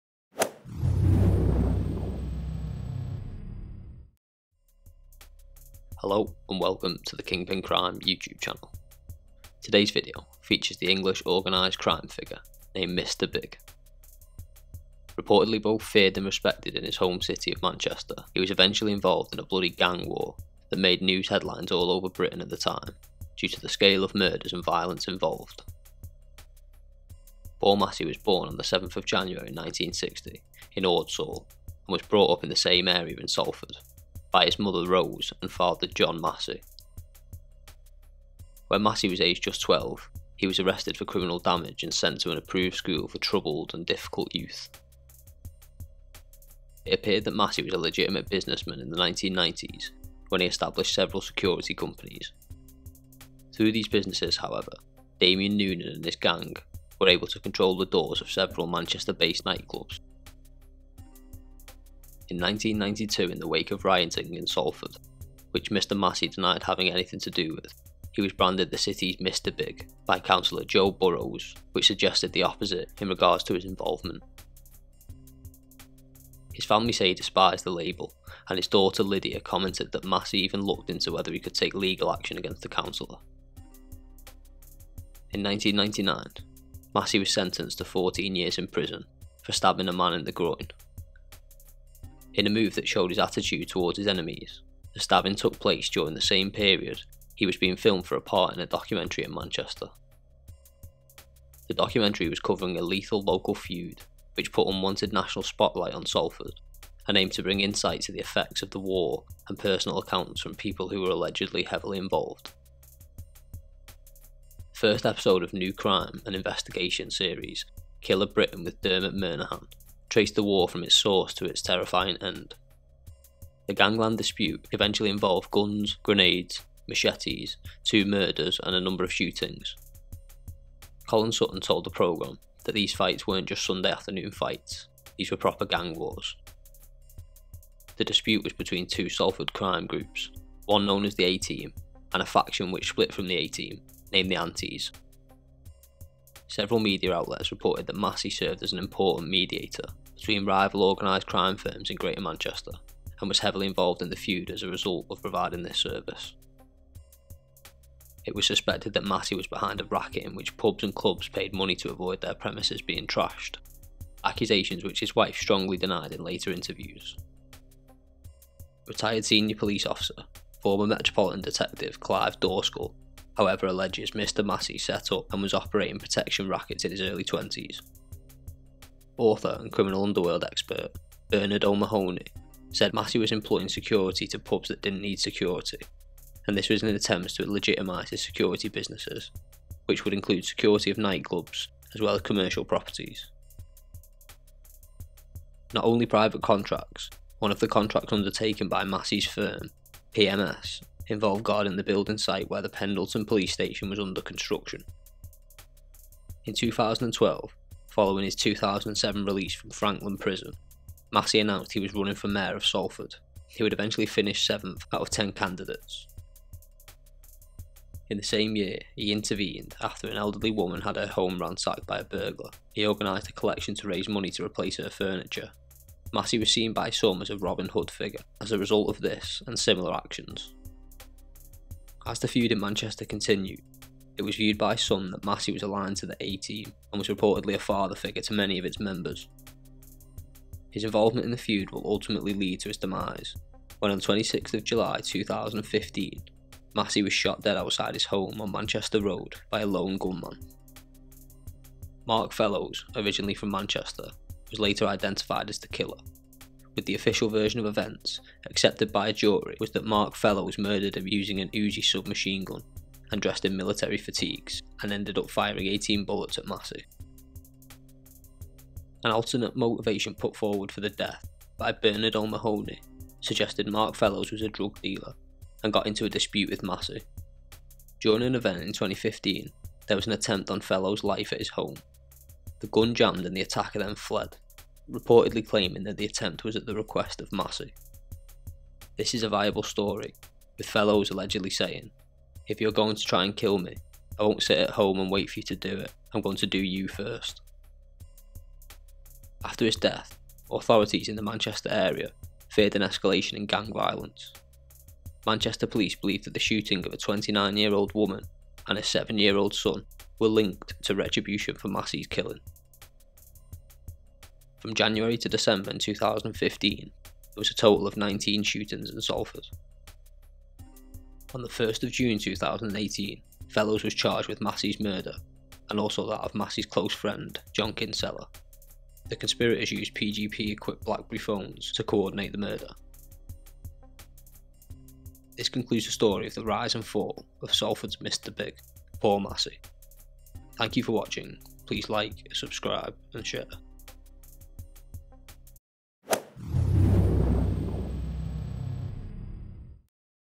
Manchester's "Mr Big" - Paul Massey [Mini Documentary]
The Story of Manchester Based Gangster Dubbed "Mr Big" - Paul Massey. Reported to Have Led Extortion Rackets By Providing Security for Nightclubs and Commercial Properties Through Legitimate Security Companies. Later He Would Allegedly Become Involved in a Salford Gang War.

Kingpin Crime